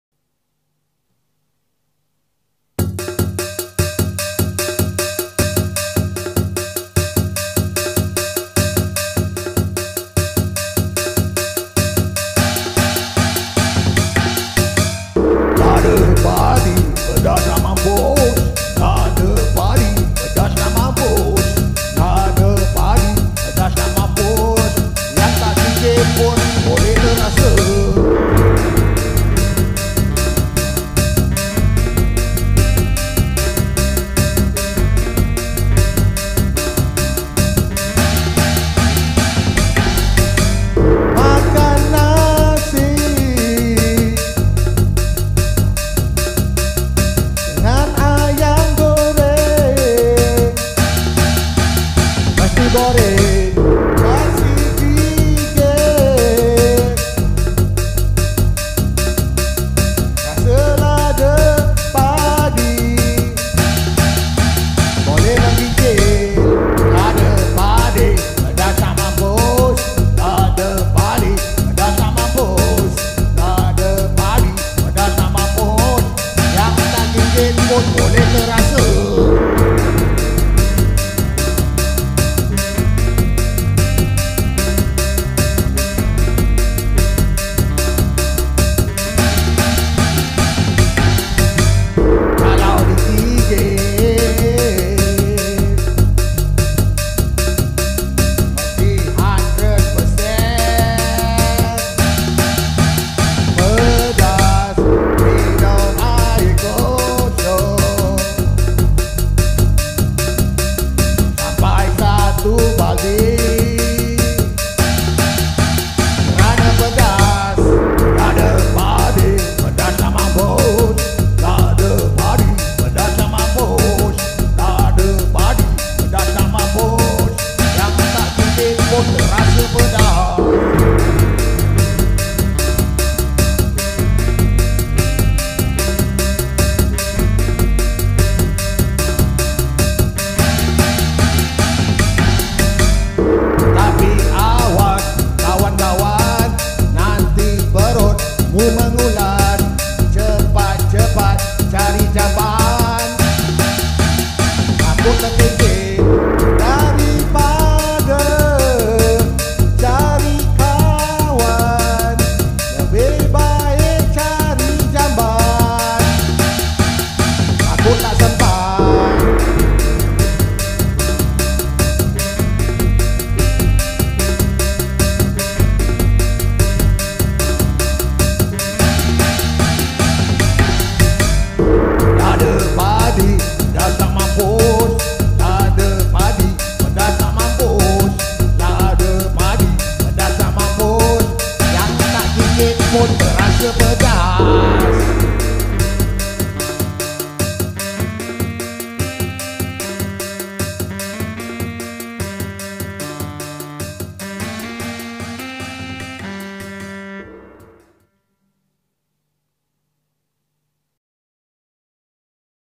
Lada Padi Pedas Nak Mampos by DDC (Monyet tak boleh makan buah Tu)
Monyet tak boleh makan cili